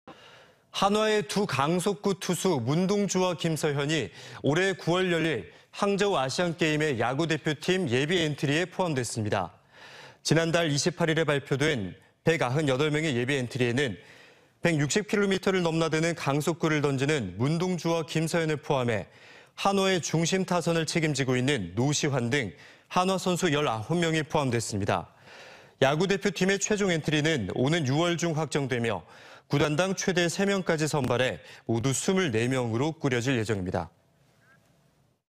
한화 문동주·김서현, 항저우 아시안게임 예비엔트리｜ TJB 대전·세종·충남뉴스
한화의
두 강속구 투수
문동주와 김서현이
올해 9월 열릴
항저우 아시안게임의
야구대표팀
예비엔트리에
포함됐습니다.

지난달(4월) 28일에 발표된
198명의 예비엔트리에는
'160km'를 넘나드는
강속구를 던지는 문동주와 김서현을 포함해
한화의
중심타선을 책임지고 있는 노시환 등
한화 선수 19명이 포함됐습니다.

야구대표팀의 최종 엔트리는
오는 6월 중 확정되며
구단당 최대 3명까지 선발해
모두 24명으로 꾸려질 예정입니다.